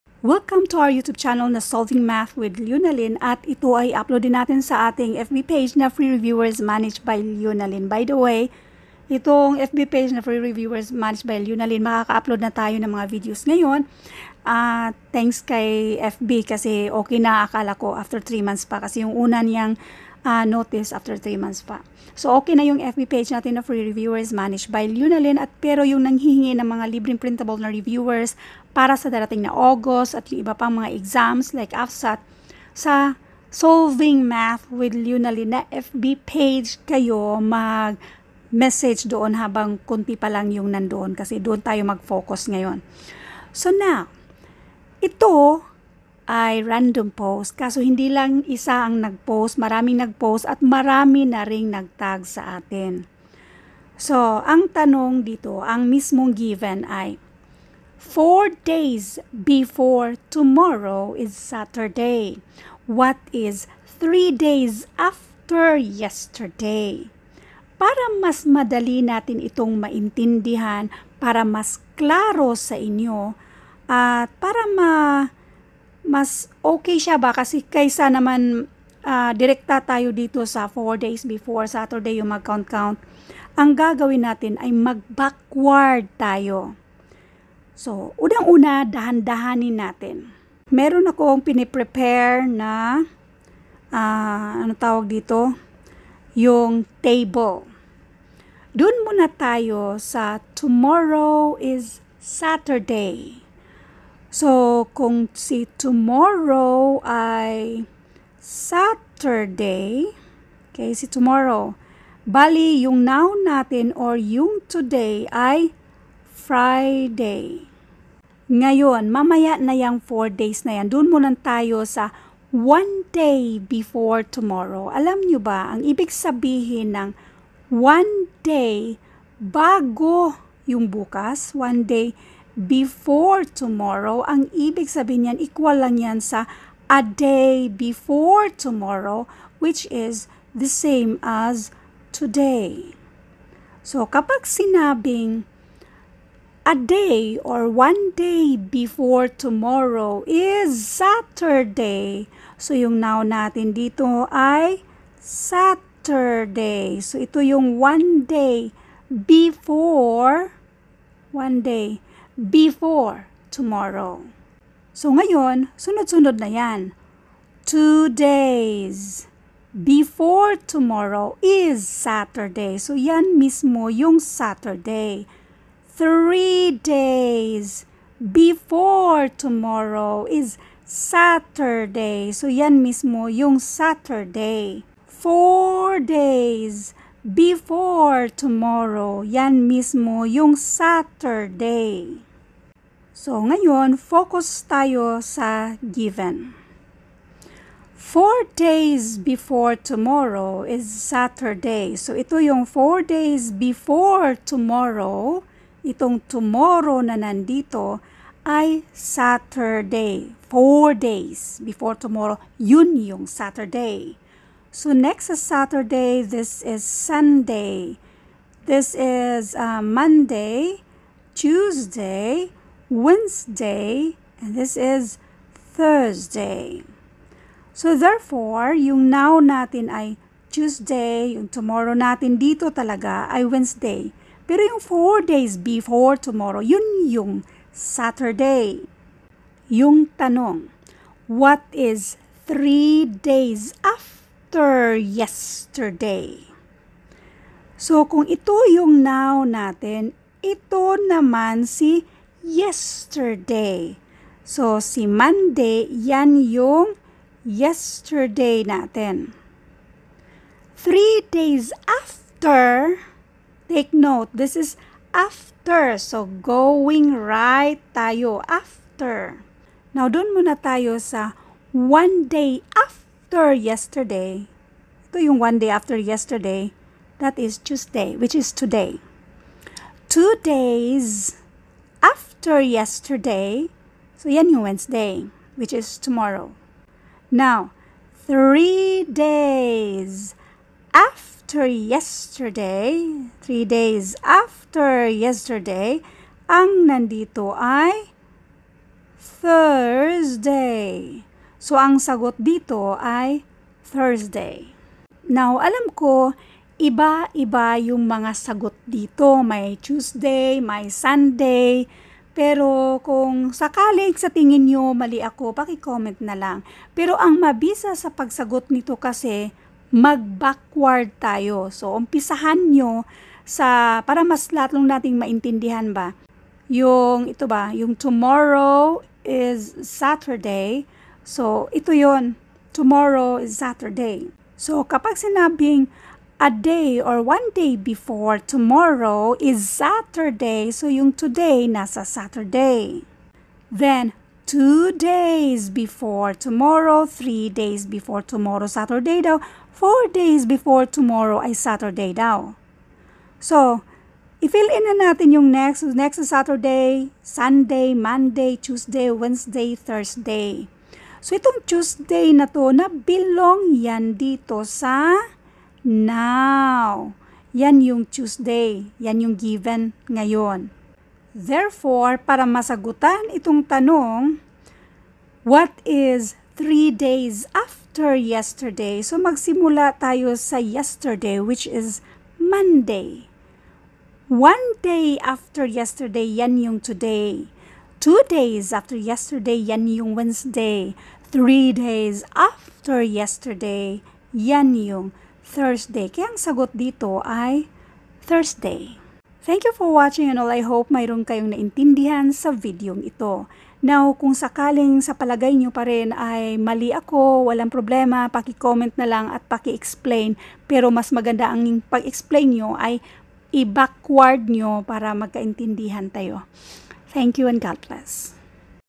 If four days before tomorrow is Saturday, what is three days after yesterday? | Logical
If four days before tomorrow is Saturday, what is three days after yesterday?
a. Monday
b. Tuesday
c. Wednesday
d. Thursday
e. Friday